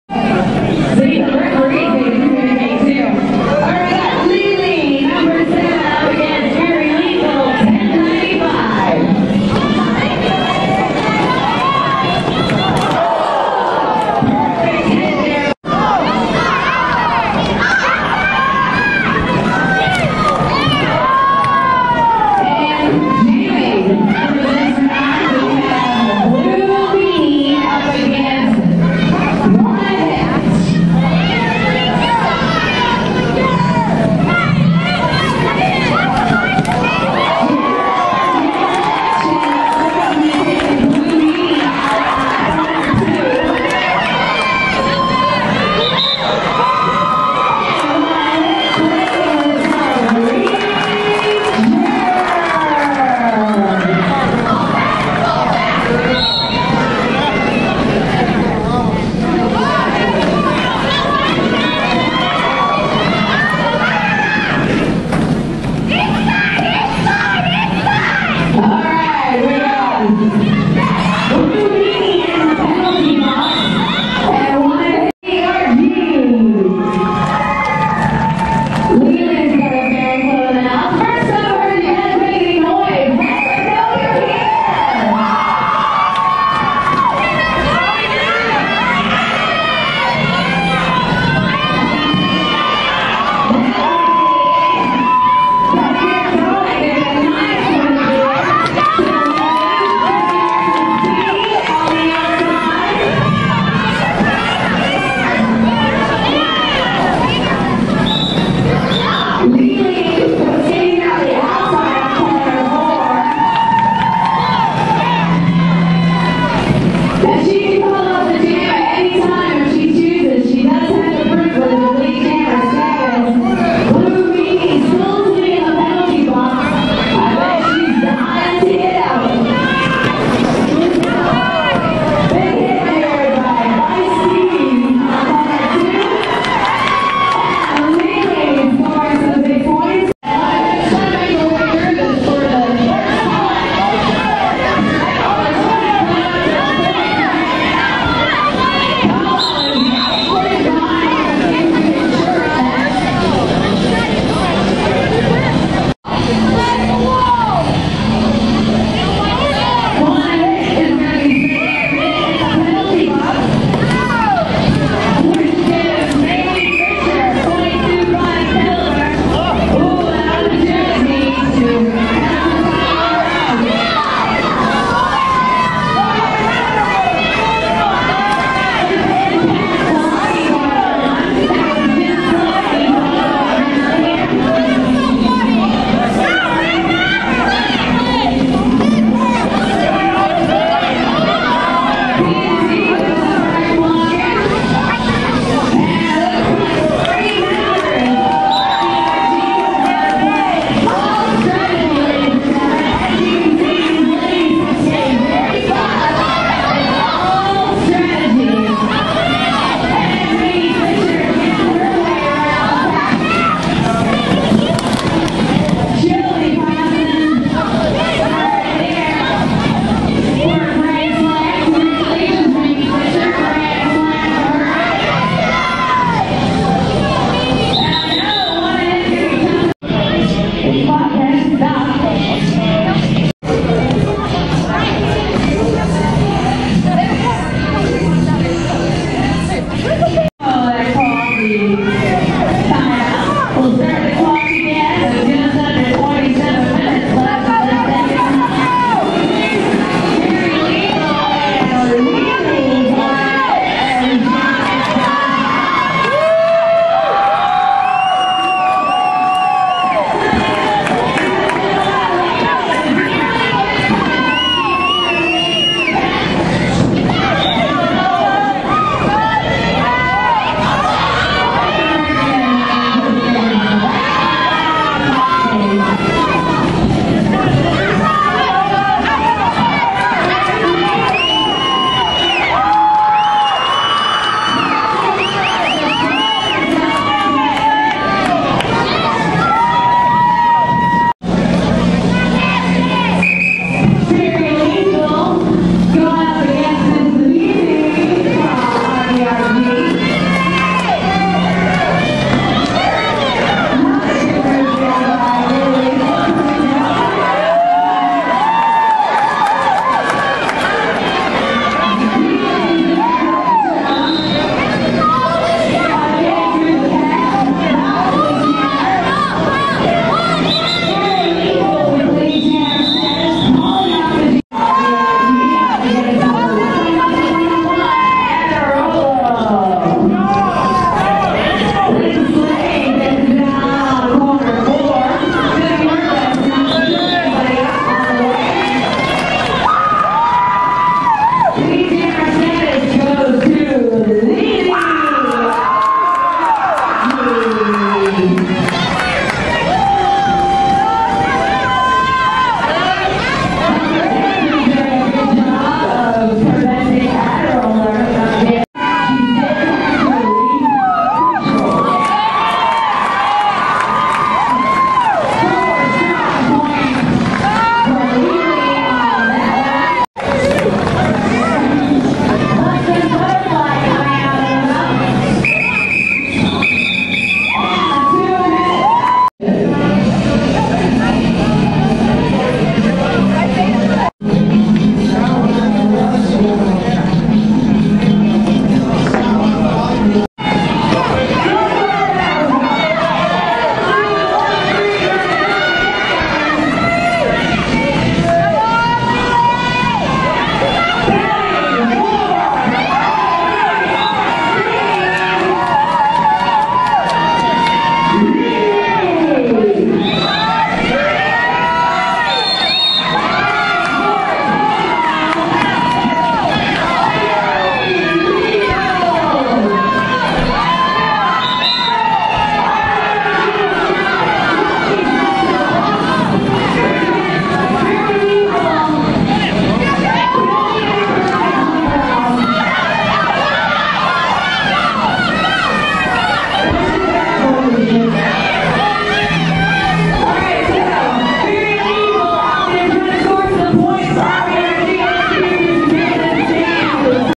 River Valley Roller Girls
RVRG  VS COMO Paint The Rink Pink footage from 10-3-09